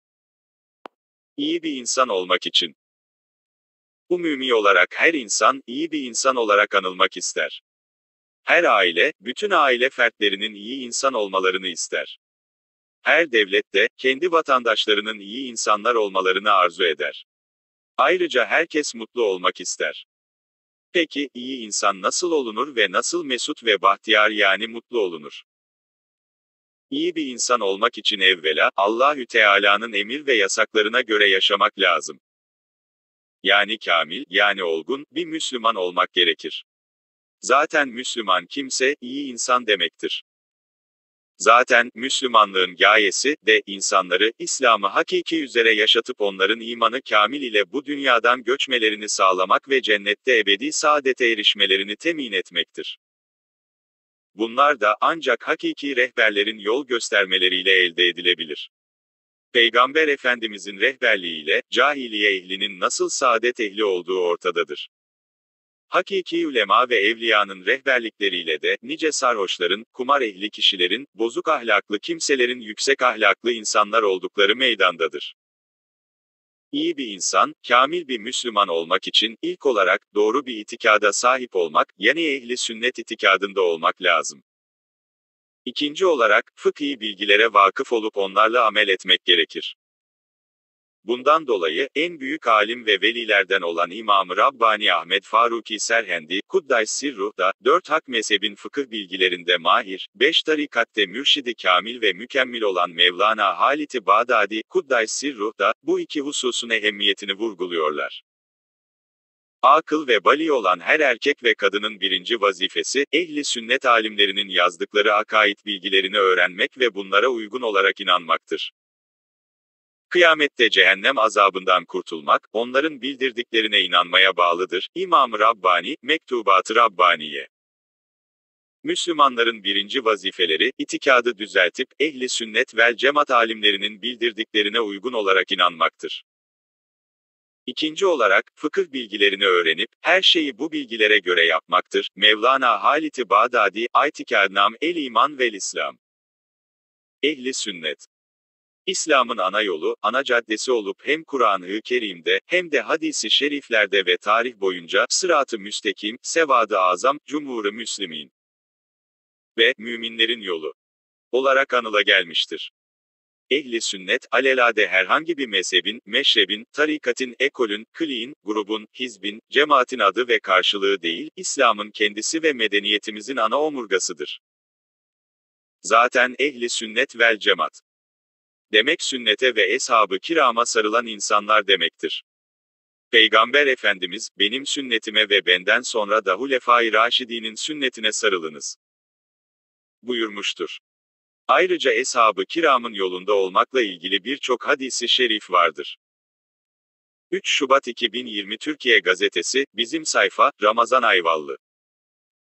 İyi bir insan olmak için - Sesli dini makale
Umûmî olarak her insan, iyi bir insan olarak anılmak ister.
Her âile, bütün âile fertlerinin iyi insan olmalarını ister.
Her devlet de, kendi vatandaşlarının iyi insanlar olmalarını arzu eder.
Ayrıca herkes mutlu olmak ister. Peki, iyi insan nasıl olunur ve nasıl mesut ve bahtiyâr yani mutlu olunur?
İyi bir insan olmak için evvelâ, Allahü teâlânın emir ve yasaklarına göre yaşamak lâzım. Yani kâmil [yani olgun] bir Müslümân olmak gerekir. Zâten Müslümân kimse, iyi insan demektir.
Zâten “Müslümânlığın gâyesi” de, “insanları, İslâm-ı hakîkî üzere yaşatıp onların îmân-ı kâmil ile bu dünyâdan göçmelerini sağlamak ve Cennet’te ebedî saadete erişmelerini temîn etmektir.”
Bunlar da, ancak hakîkî rehberlerin yol göstermeleriyle elde edilebilir. Peygamber Efendimizin rehberliğiyle, câhiliye ehlinin nasıl saadet ehli olduğu ortadadır. Hakîkî ulemâ ve evliyânın rehberlikleriyle de, nice sarhoşların, kumar ehli kişilerin, bozuk ahlâklı kimselerin yüksek ahlâklı insanlar oldukları meydândadır.
İyi bir insan, kâmil bir Müslümân olmak için, ilk olarak, doğru bir itikâda sâhip olmak, yanî Ehl-i Sünnet itikâdında olmak lâzım. İkinci olarak, fıkhî bilgilere vâkıf olup onlarla amel etmek gerekir.
Bundan dolayı, en büyük âlim ve velîlerden olan İmâm-ı Rabbânî Ahmed Fârûkî Serhendî (kuddise sirruh) da, dört hak mezhebin fıkıh bilgilerinde mâhir, beş tarîkatte mürşid-i kâmil ve mükemmil olan Mevlânâ Hâlid-i Bağdâdî (kuddise sirruh) da, bu iki husûsun ehemmiyetini vurguluyorlar.
“Âkıl ve bâliğ olan her erkek ve kadının birinci vazîfesi, Ehl-i Sünnet âlimlerinin yazdıkları akâid bilgilerini öğrenmek ve bunlara uygun olarak inanmaktır. Kıyâmette Cehennem azâbından kurtulmak, onların bildirdiklerine inanmaya bağlıdır.” [İmâm-ı Rabbânî, Mektûbât-ı Rabbâniyye]
“Müslümânların birinci vazîfeleri, itikâdı düzeltip, Ehl-i Sünnet vel-cemâat âlimlerinin bildirdiklerine uygun olarak inanmaktır. İkinci olarak, fıkıh bilgilerini öğrenip, her şeyi bu bilgilere göre yapmaktır.” [Mevlânâ Hâlid-i Bağdâdî, İ’tikâdnâme/el-Îmân ve’l-İslâm]
Ehl-i Sünnet; İslâm’ın ana yolu, ana caddesi olup hem Kur’ân-ı kerîmde, hem de hadîs-i şerîflerde ve târih boyunca “sırât-ı müstekîm”, "sevâd-ı a'zam", "cumhûr-ı müslimîn" ve "müminlerin yolu" olarak anılagelmiştir.
"Ehl-i Sünnet", alelâde herhangi bir mezhebin, meşrebin, tarîkatin, ekolün, kliğin, grubun, hizbin, cemâatin adı ve karşılığı değil, İslâm’ın kendisi ve medeniyetimizin ana omurgasıdır.
Zâten "Ehl-i Sünnet vel-cemâat" demek Sünnete ve Eshâb-ı kirâma sarılan insanlar demektir. Peygamber Efendimiz, "Benim sünnetime ve benden sonra da Hulefâ-i Râşidînin sünnetine sarılınız" buyurmuştur. Ayrıca Eshâb-ı kirâmın yolunda olmakla ilgili birçok hadîs-i şerîf vardır.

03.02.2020 Türkiye Gazetesi, Bizim Sayfa, Ramazan Ayvallı